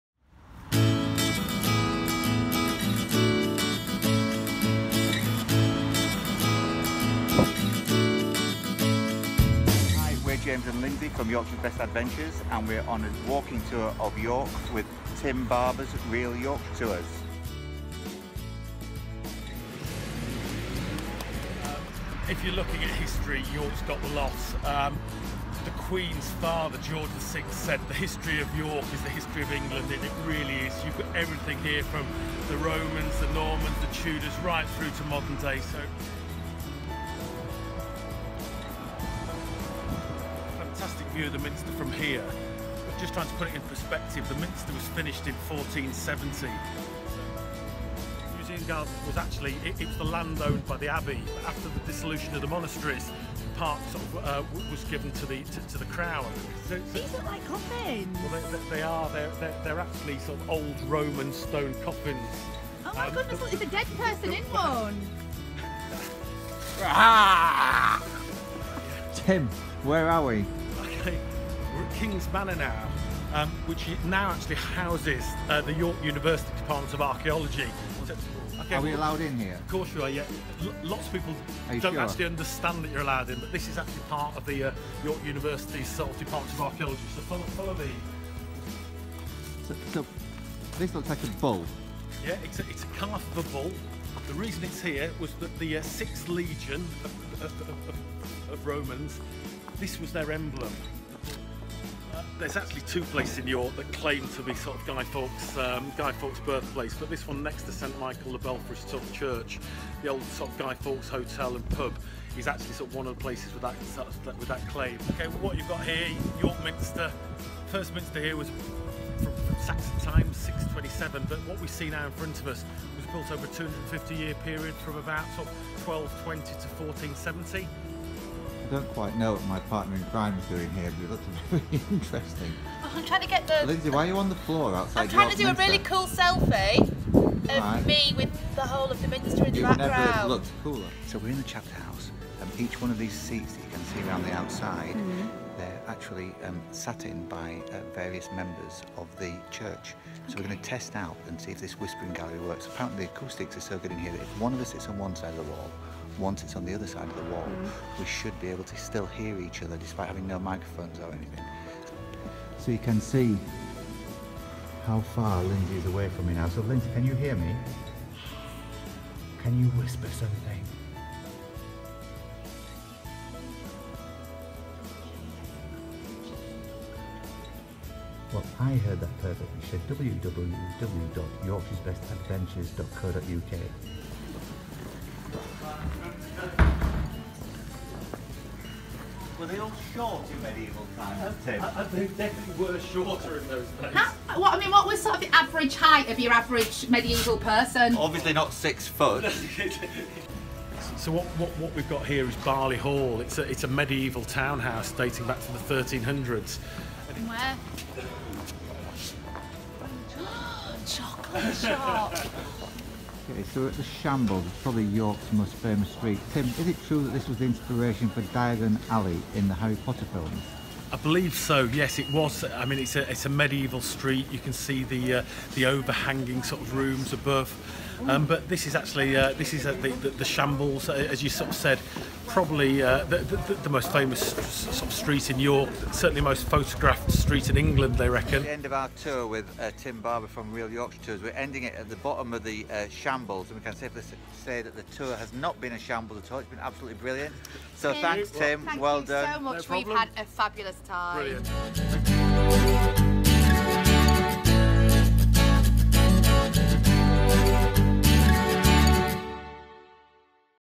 Vlog: York Walking Tour with Real Yorkshire Tours
Join James and Lyndsey of Yorkshire's Best Adventures on a walking tour of York with Blue Badge guide Tim Barber of Real Yorkshire Tours.